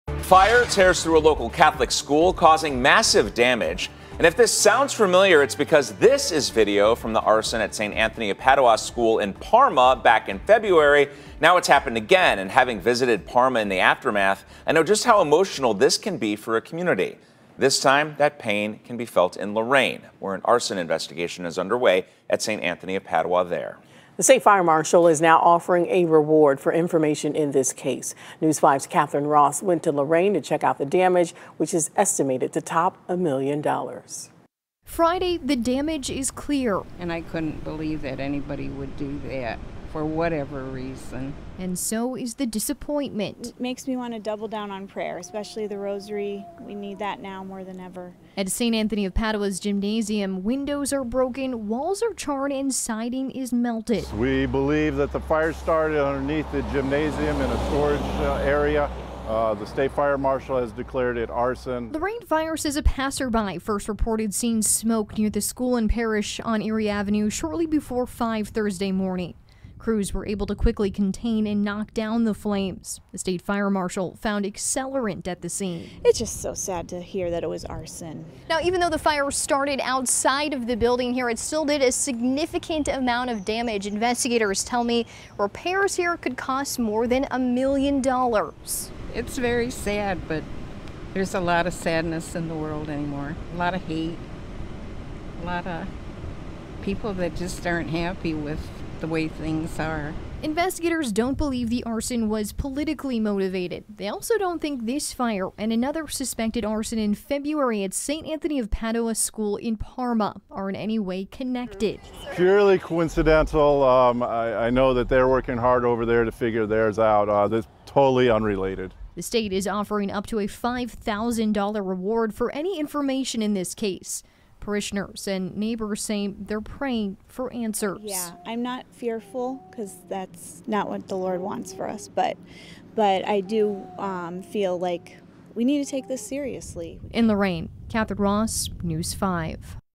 Suspected arson fire causes more than $1 million in damage to St. Anthony of Padua in Lorain
Suspected arson fire at St. Anthony of Padua in Lorain